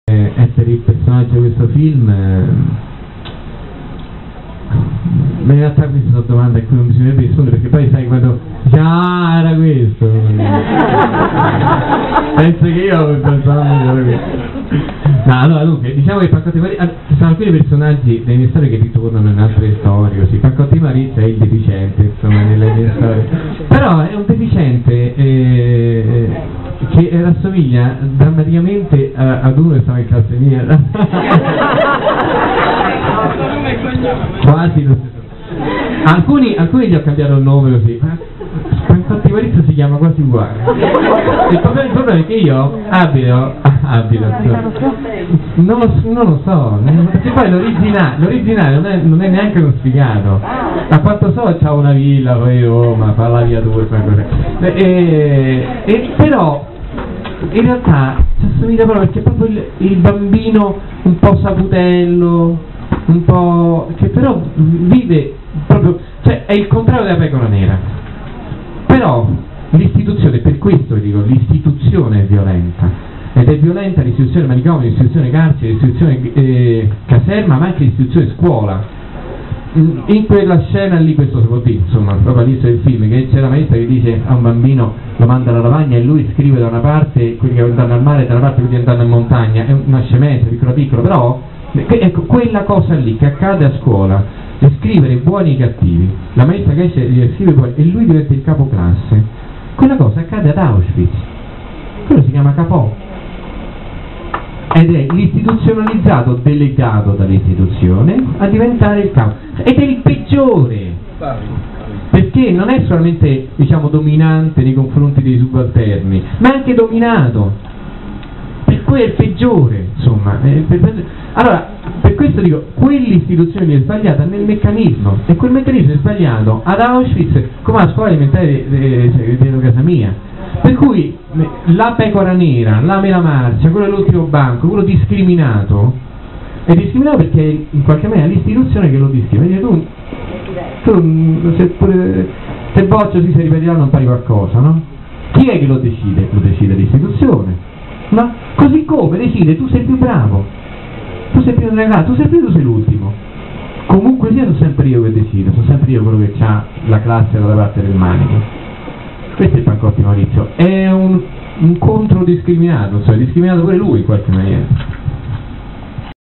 Pancotti Maurizio ne La pecora nera
- 05/10/2010 - Cinema ABC di Bari - Ascanio Celestini spiega il personaggio di Pancotti Maurizio nel film. Ad intervistarlo Antonella Gaeta, critico cinematografico, giornalista di Repubblica e selezionatrice dell'ultima Mostra del Cinema di Venezia.